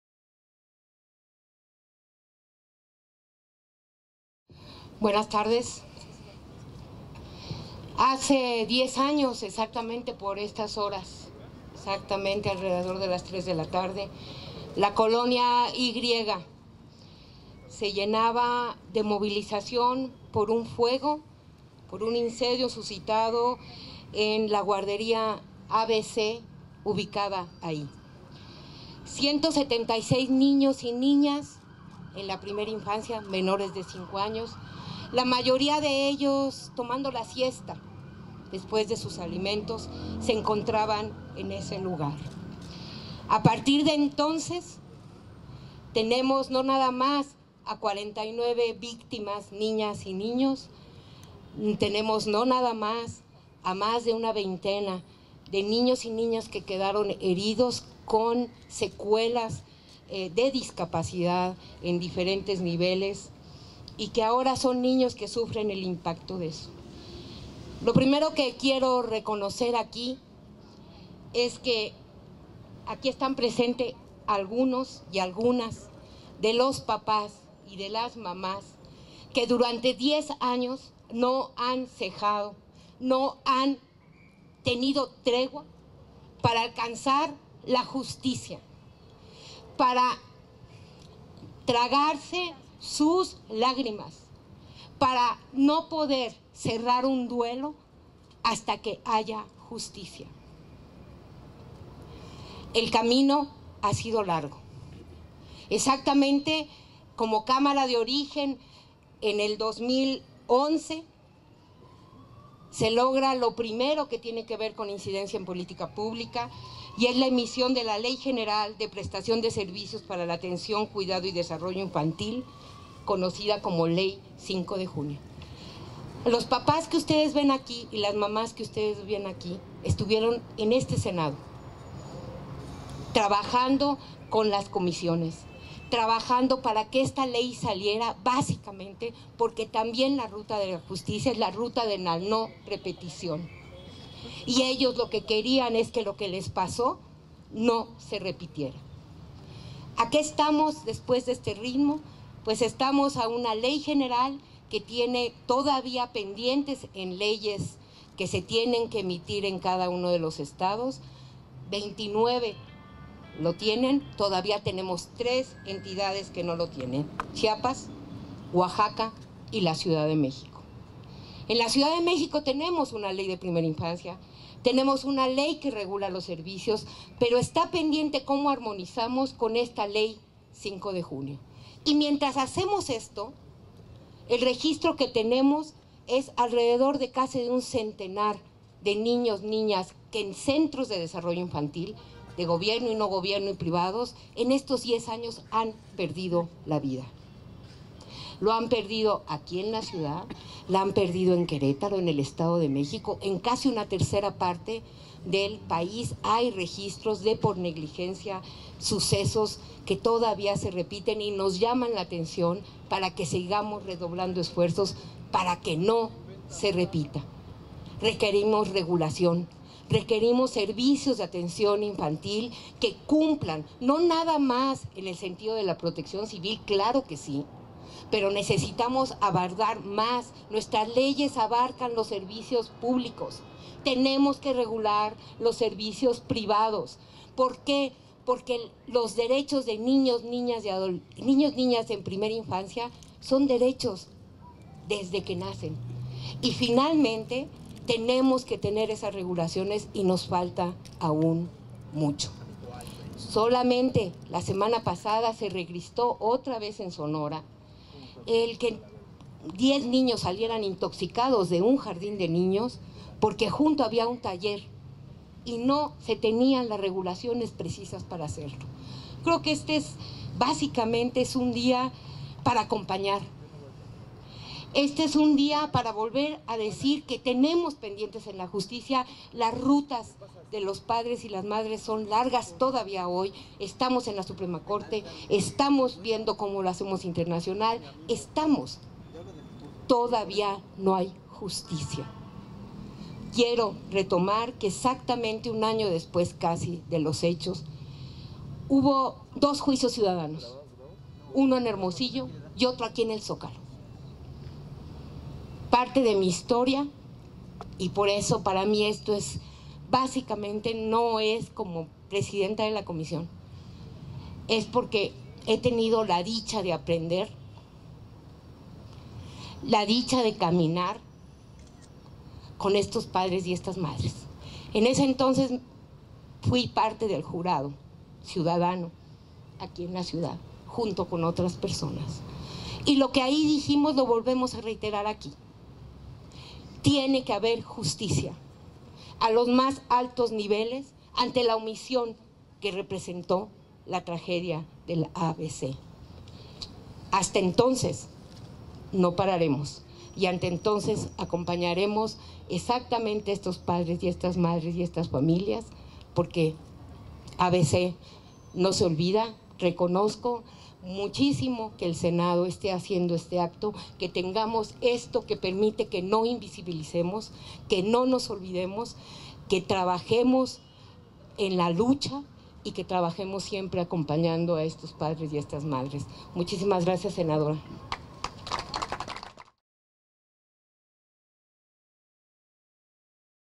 Discurso Nasheli Ramírez en la instalación fotográfica 10 años ABC, en el Senado
Discurso Nasheli Ramírez, Presidenta de CDHDF, en la instalación fotográfica 10 años ABC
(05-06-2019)